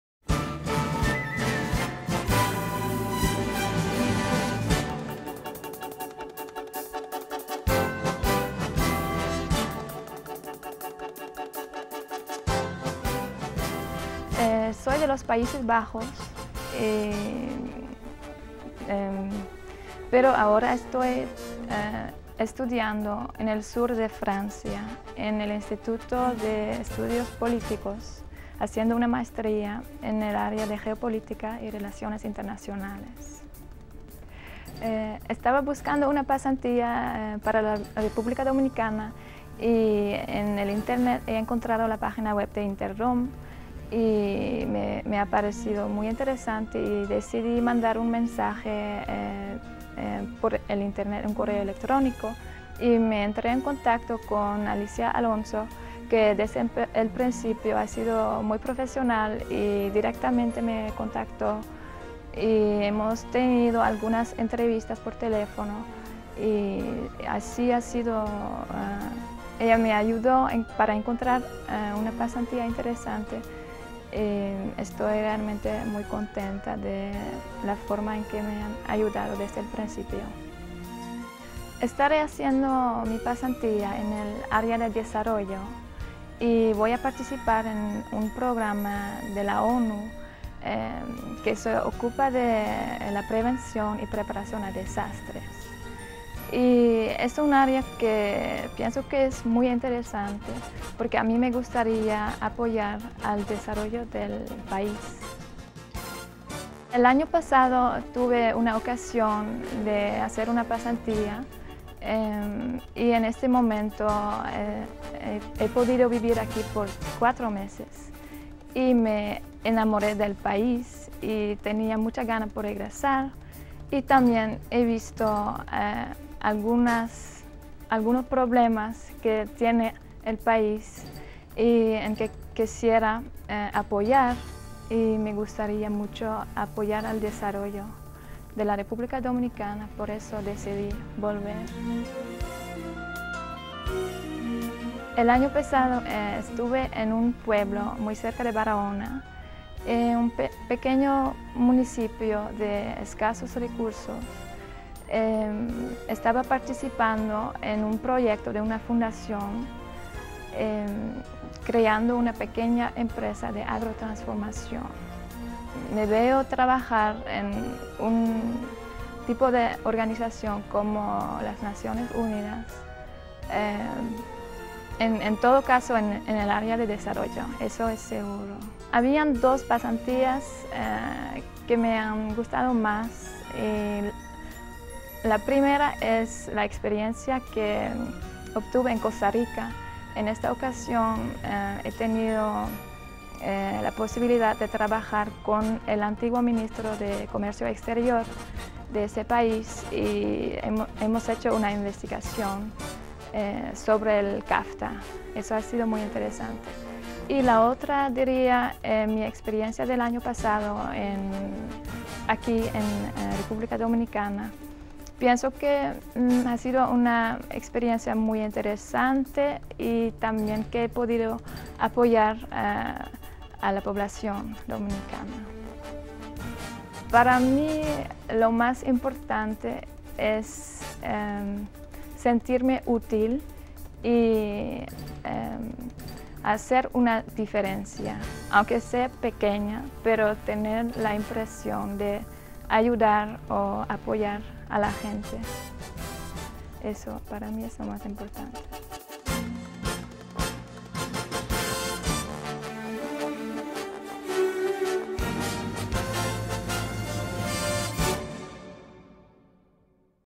GFDD / InteRDom - Masha Wisman (Pasante InteRDom / InteRDom Intern)
Interview with InteRDom Student. he program is aimed at selected applicants who are eager to integrate into Dominican society. InteRDom participants will act as representatives of their country in both the academic and professional world. We also ensure that each program is tailor-made to suit each applicant's individual needs.